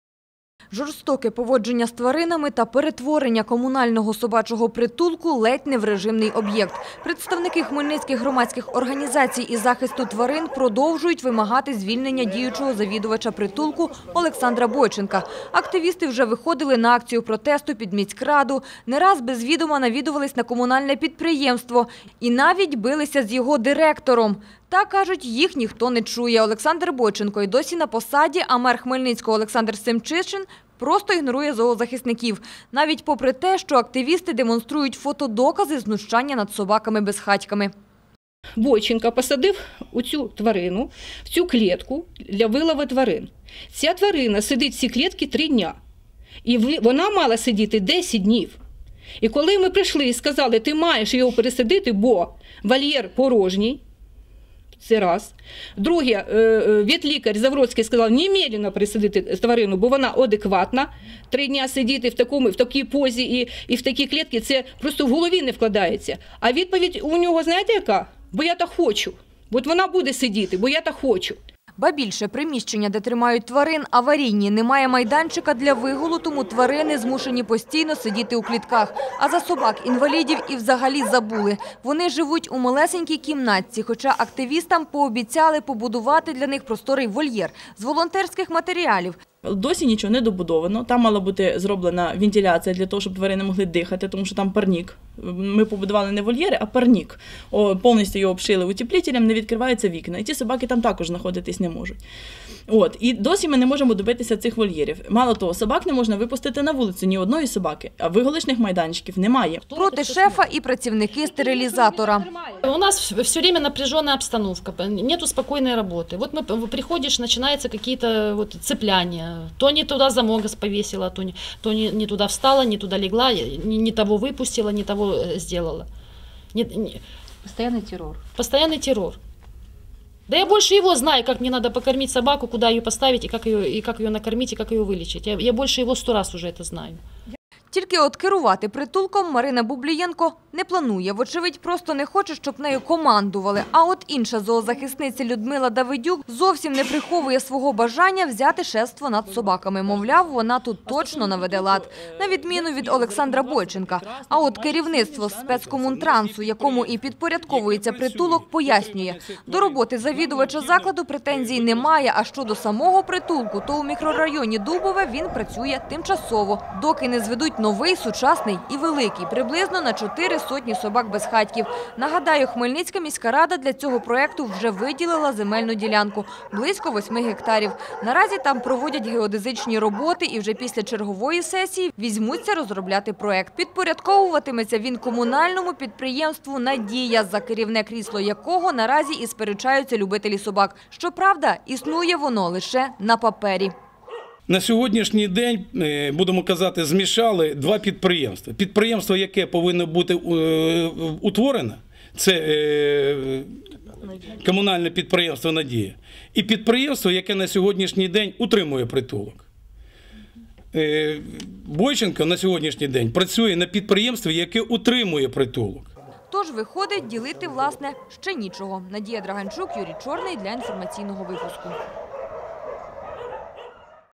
Як у Хмельницькому лаються за крісло шефа над собаками
Хмельницькі зоозахисники проти завідувача хмельницького стерилізатора для тварин та влади. Представник громадських організацій «SOS» та «Врятуй життя» вимагають звільнити директора комунального підприємства «Надія» Олександра Бойченка. Кажуть, він не любить ані собак, ані людей. Тільки от «Надії» немає. Комунальне підприємство з такою назвою  у Хмельницькому створили лише на папері. То що ж ділять зоозахисники, - дізнавалися наші журналісти.